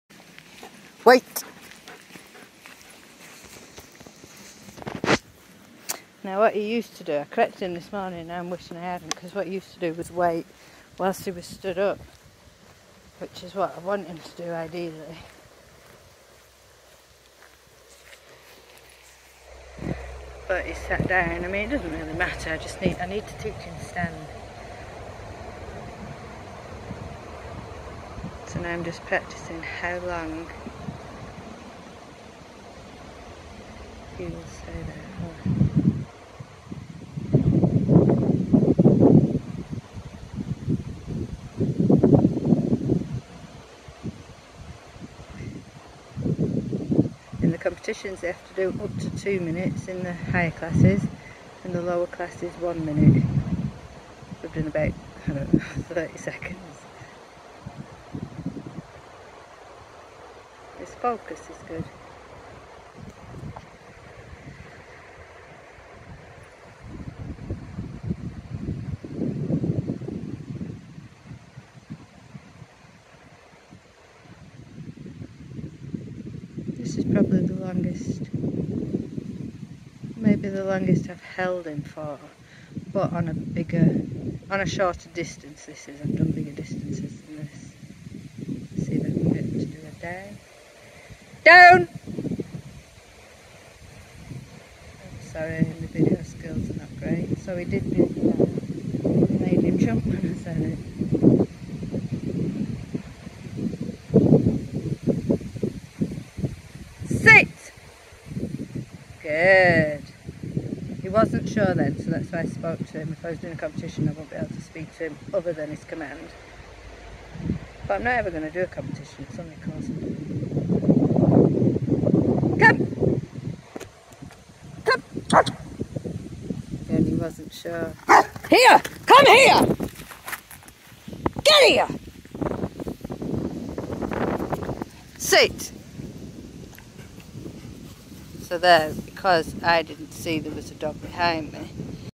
Practicing for a 2 minute wait, competitive dog obedience
Distance control, 2 minutes
wait, down, sit

and the end went wrong because someone let their dog come close to me, just as I released my dog.

People please, if you see someone training their dog, stay out of their space especially when your not even passing through where I was, the owner was stood about 30 foot away but had allowed his dog to come about 6 foot away from me.

Anyway my dog saw to it and moved it back by charging it.

I don't know how many times I need to say this but dogs do not like other dogs in their space, especially when they don't know them!!!